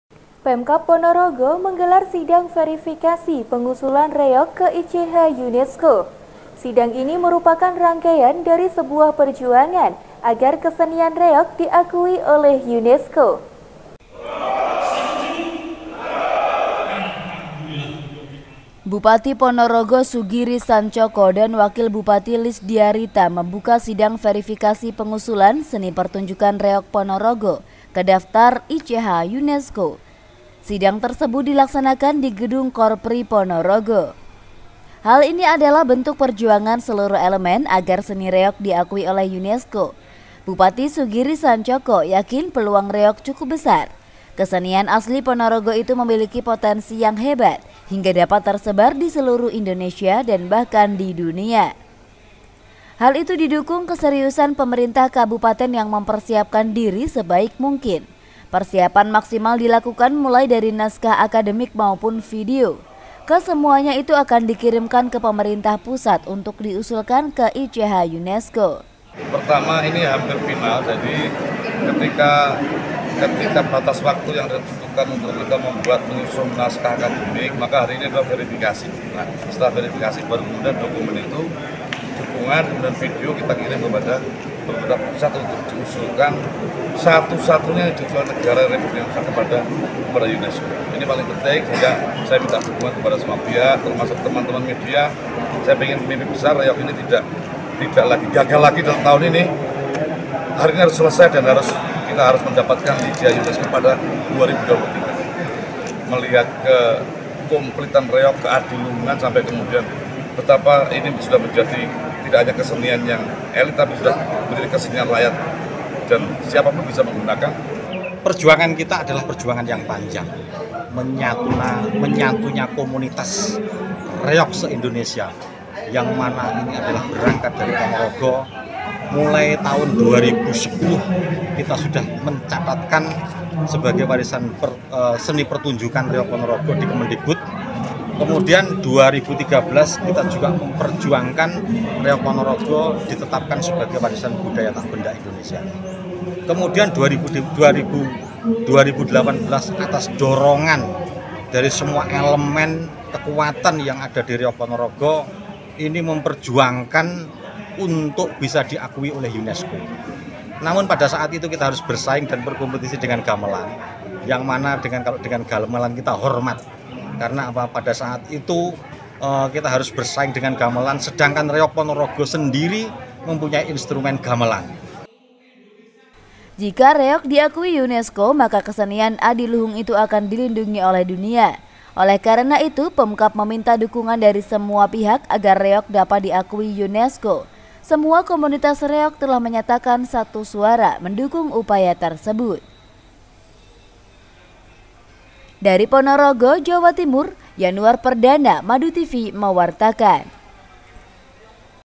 Sidang Verifikasi Pengusulan Reog ke ICH UNESCO
Ponorogo – Bupati Ponorogo Sugiri Sancoko dan Wakil Bupati Ponorogo Lisdyarita membuka sidang verifikasi pengusulan seni pertunjukan reog ponorogo ke daftar Intangible cultural heritage (ICH) UNESCO. Sidang tersebut dilaksanakan di Gedung Korpri Ponorogo.

Hal itu sebagai bentuk perjuangan seluruh elemen agar seni Reog diakui oleh UNESCO nesco. Kang Bupati Sugiri yakin peluang Reog cukup besar.

Kesenian asli Ponorogo itu memiliki potensi yang hebat hingga dapat tersebar di seluruh Indonesia dan bahkan di dunia.

Hal itu didukung keseriusan Pemkab yang mempersiapkan diri sebaik mungkin. Persiapan maksimal dilakukan mulai dari naskah akademik maupun video. Kesemuanya akan dikirimkan ke pemerintah pusat untuk diusulkan ke ICH UNESCO.

Jika Reog diakui UNESCO, maka kesenian Adiluhung itu akan dilindungi oleh dunia. Oleh karena itu, Pemkab meminta dukungan dari semua pihak agar Reog dapat diakui UNESCO. Sedangkan semua komunitas Reog juga telah menyatakan satu suara mendukung upaya tersebut . (yp)